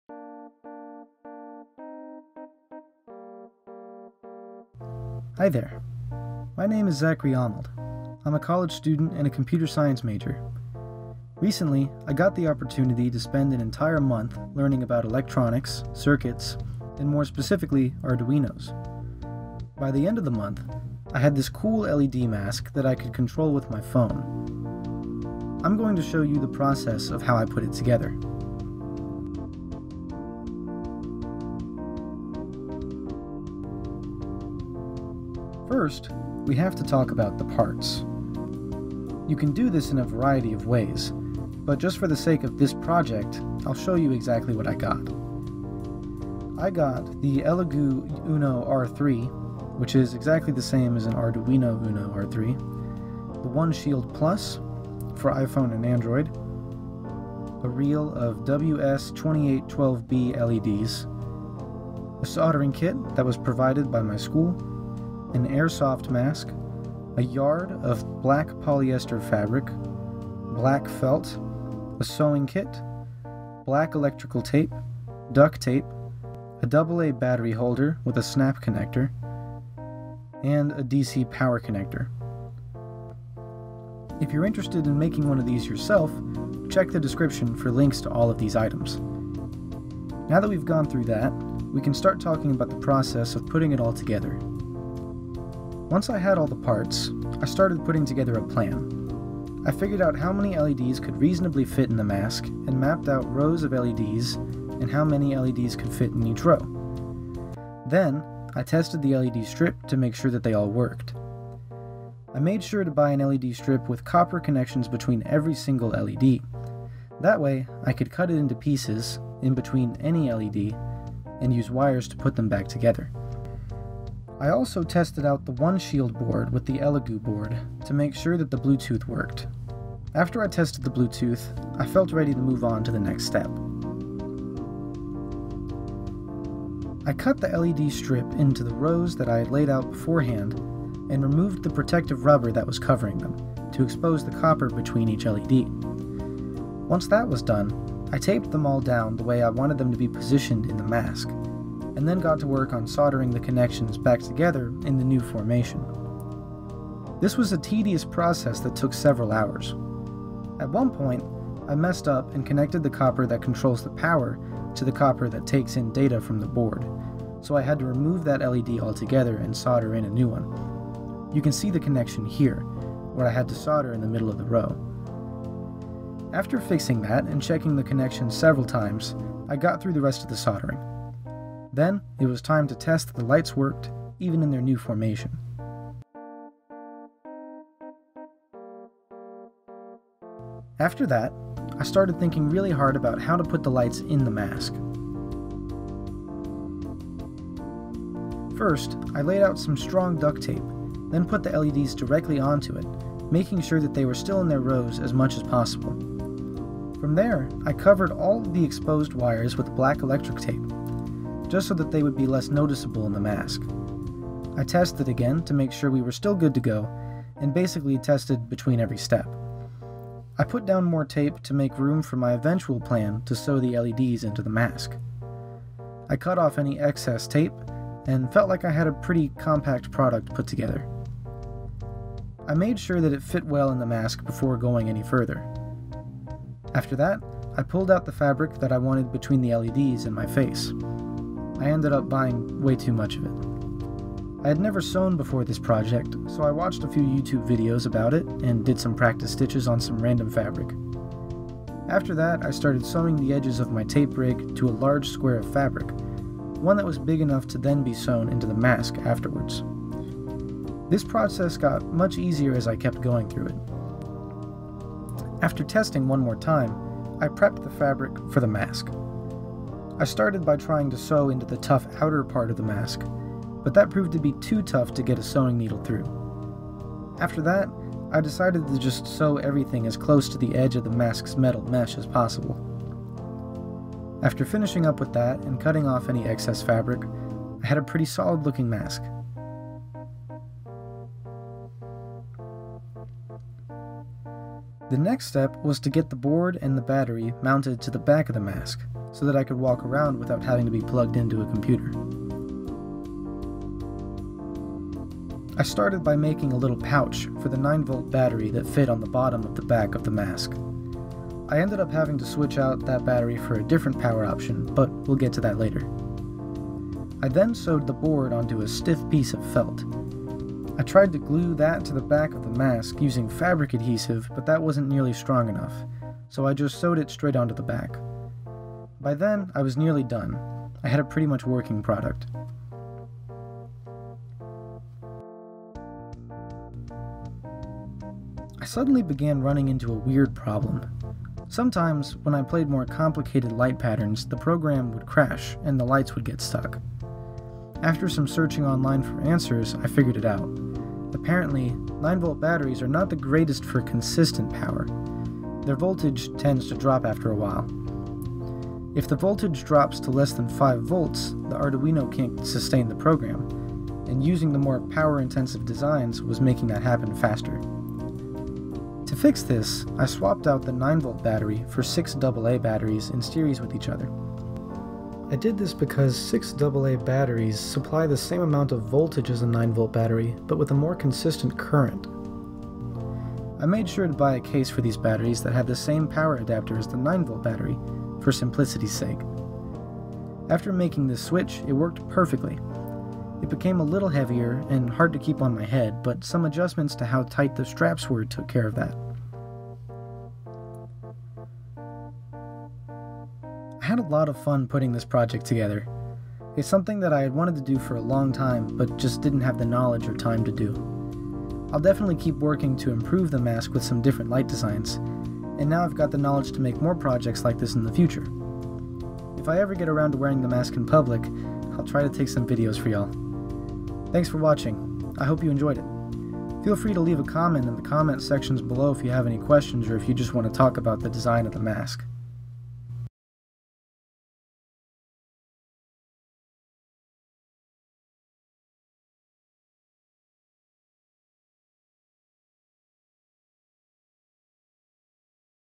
Making an LED Mask with Bluetooth
This video explains the process of putting together an LED Mask, and how I went about it.
OPEN DESCRIPTION FOR GIT REPOSITORY AND EQUIPMENT LIST

I hope you enjoyed seeing my process for this project, and if you're thinking about making one yourself I hope it helped!

Here's a link to the git repository with my code for this project in it:

Here's a list of links to where I got the equipment:

Elegoo Uno R3 & DC Power Connector

Soldering Kit

Airsoft Mask

Black Felt

Black Electric Tape

Duct Tape

6 AA Battery Holder